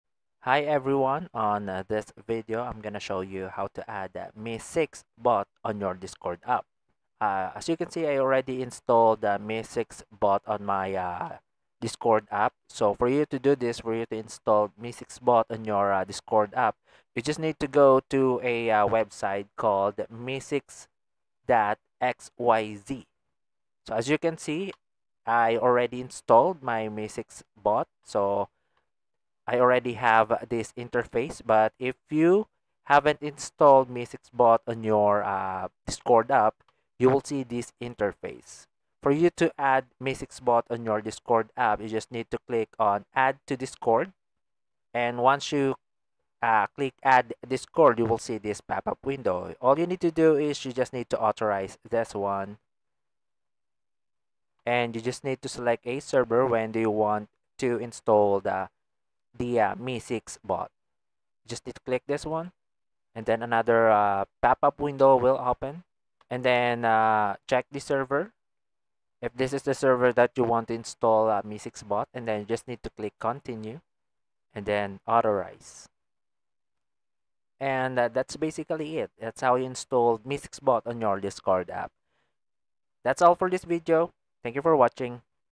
How to Add MEE6 Bot to Discord
In this video, I'll show you how to add the MEE6 Bot to your Discord server.